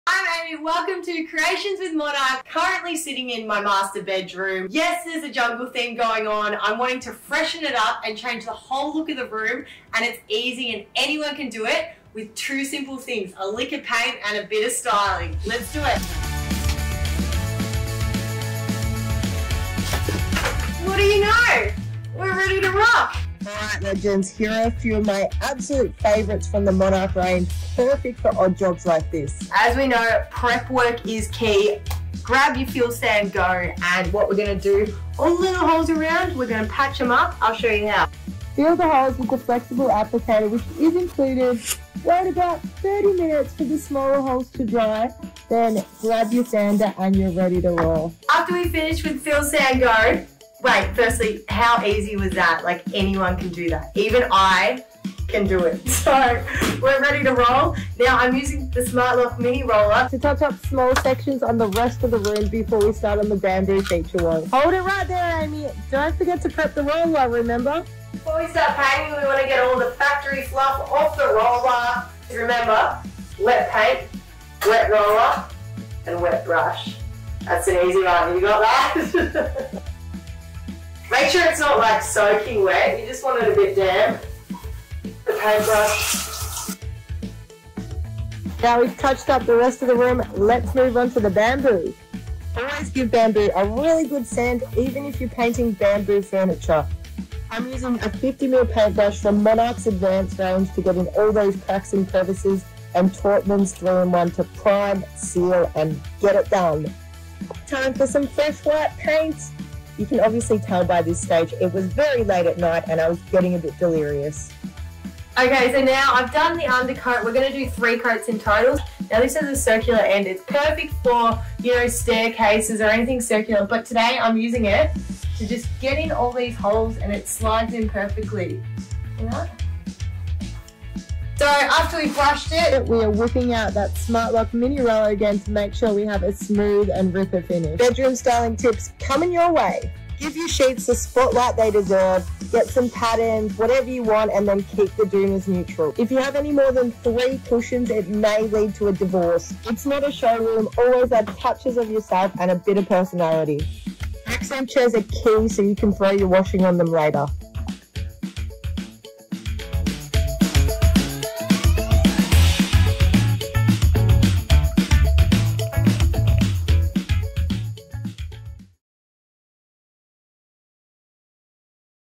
Aimee's Bedroom transformation | Creations with Monarch
🔨 Attention DIY'ers! Aimee's Bedroom transformation is now available! In this episode, Aimee transforms her bedroom from a jungle theme to a chic coastal paradise. Find out how you too can do this in your own home!